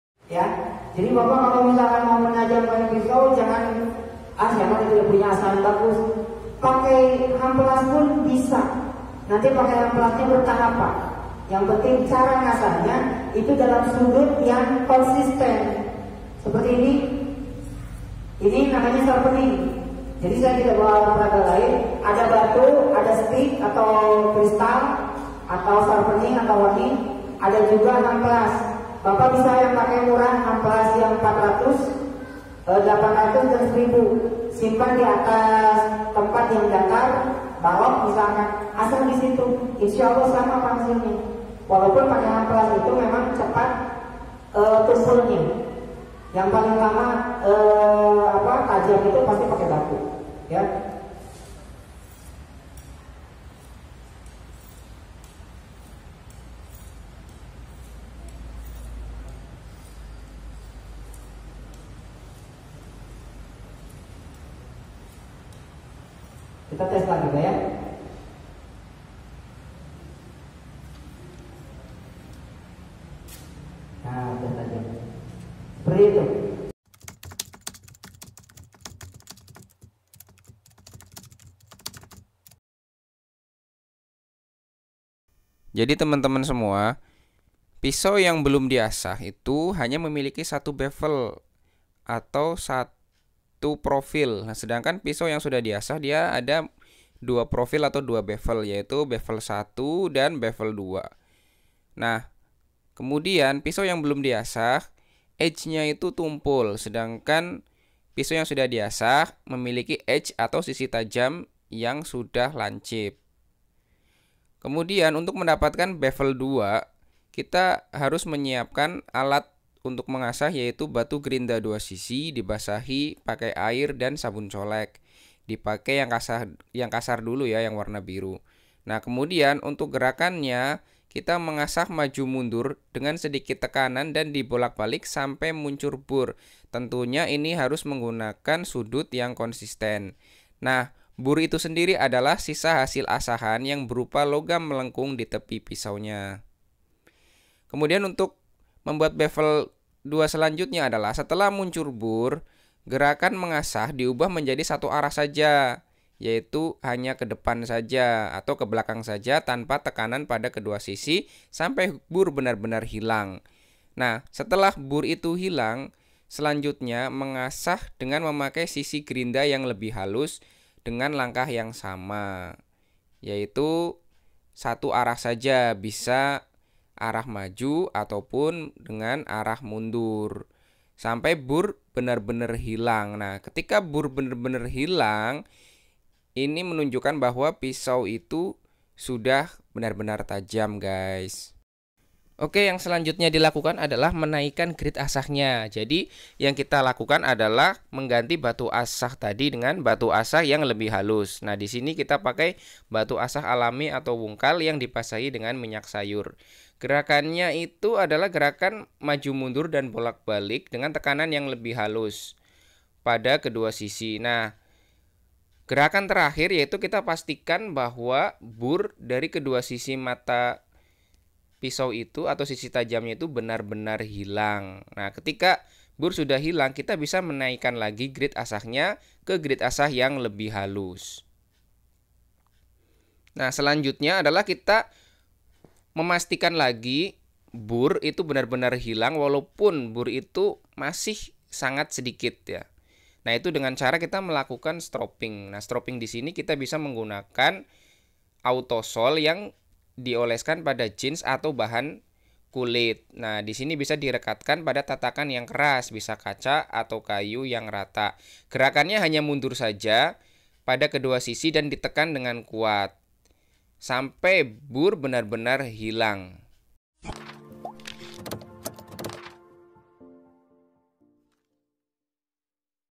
Panduan mengasah pisau - Juleha Indonesia
Bismillah...
Mengasah itu kuncinya sudut yang konsisten. Gak harus pakai asahan yang mahal...
(Juleha Indonesia)

Selamat menonton...
Tonton sampai habis videonya ya...?
Terima kasih...

Jangan lupa mampir ya Gaes...
Terima kasih...